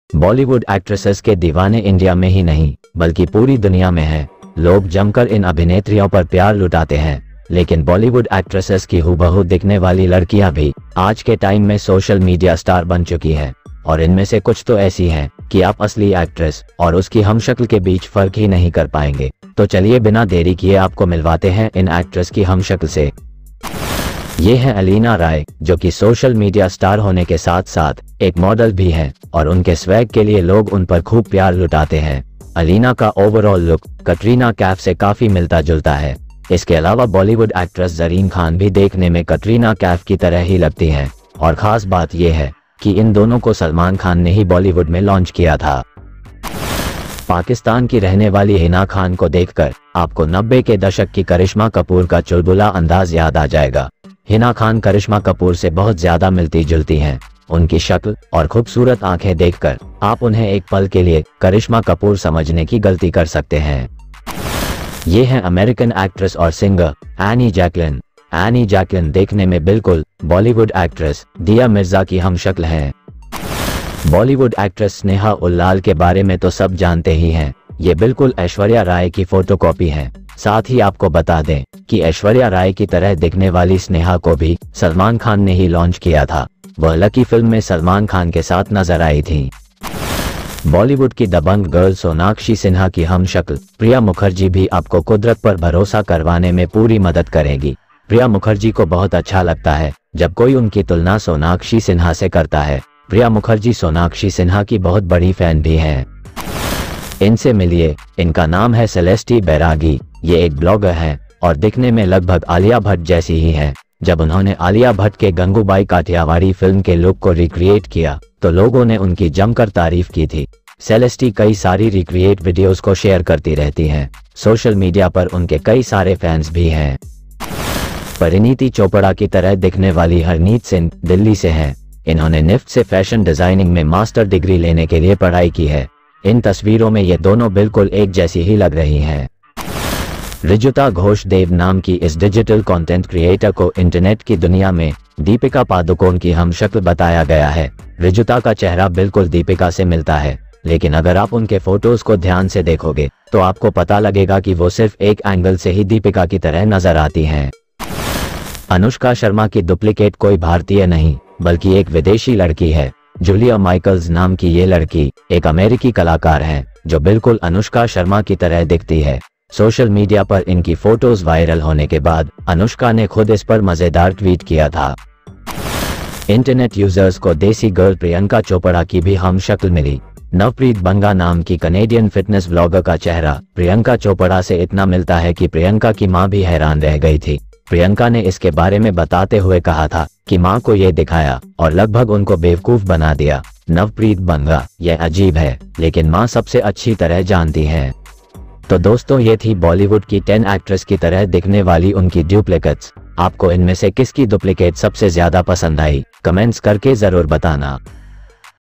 Top 10 Duplicate of Bollywood Actors - Aishwarya, Katrina, Deepika Duplicate
Imagine how would your doppelganger be!  Well, everyone of us has a twin across the globe and we so wish we could meet him/her at least once in life. For some, this might just be a myth, but with proofs surfacing over the internet, people are believing it to be the reality.
Nowadays, we come across headlines where doppelgangers of celebrities are highlighted. No doubt, we are surprised by looking at those pictures, but at the end, we need to believe that it’s the truth.

Here we are with 10 Bollywood actress look alike photographs; you are surely going to enjoy this list.

Katrina Kaif Duplicate,
Alia Bhatt Duplicate,
Karishama Kapoor Duplicate,
Diya Mirza Duplicate,
Deepika Padukone Duplicate,
Priyanka chopra Duplicate,
Parineeti Chopra Duplicate,
Sonakshi Sinha Duplicate,
Aishwarya Rai Duplicate,
Anushka Sharma Duplicate,